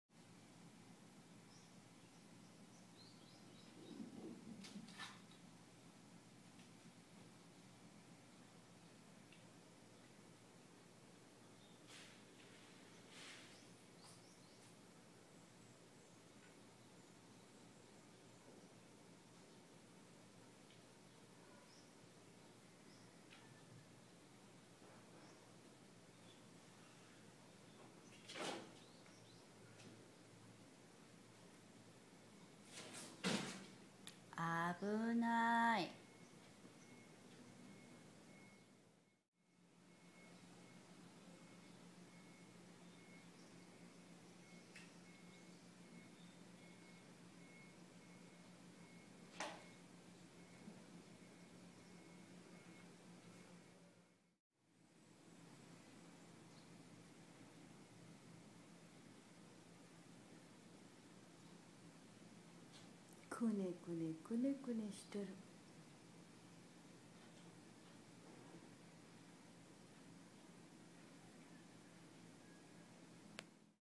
猫 縄張りを堅守するごっこ遊び
すりす〜り＆くねくね〜アクションがたまらなく可愛い猫ズ(=´▽`=)